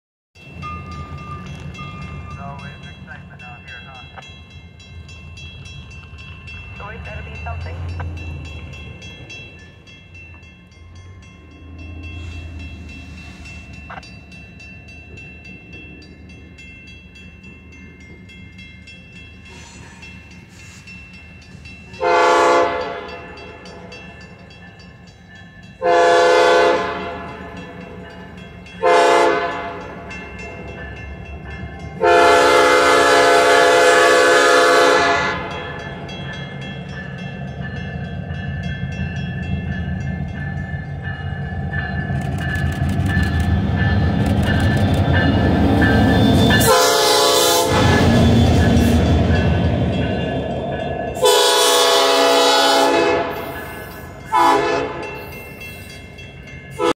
Metra 10 at Blue Island, IL
Metra 10 a rebuilt EMD GP23ECO loco runs light at the Vermont st. ata. I Blue Island, IL. Filmed on 6/23/2024